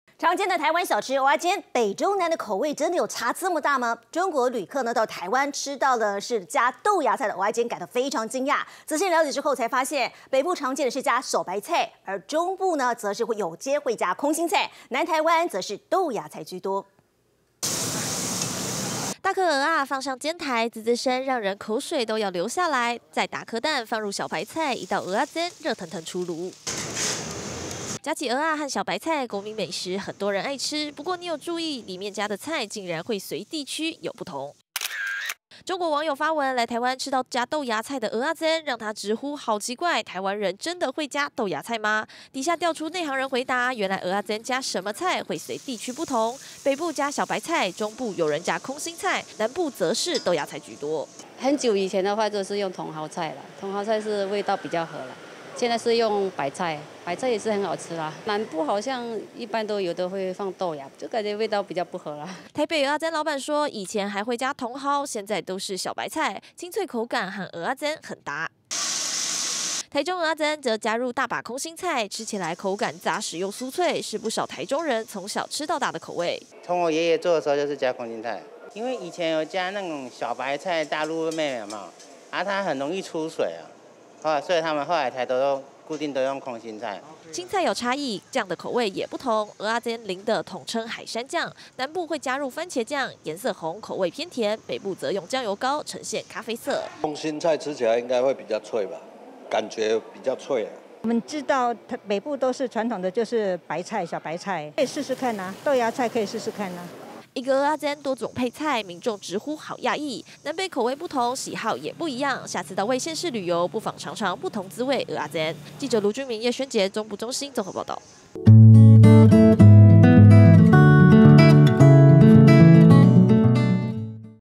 蚵仔煎「加啥菜」才對味？ 北中南各有擁護「菜」
蚵仔煎是平時常見的小吃，不過一名中國網友來台吃到加豆芽菜的蚵仔煎很驚訝，原來面加的菜，竟然會隨著北中南地區不同，口味也不同，北部常見小白菜，中部有空心菜，南部則是豆芽菜居多。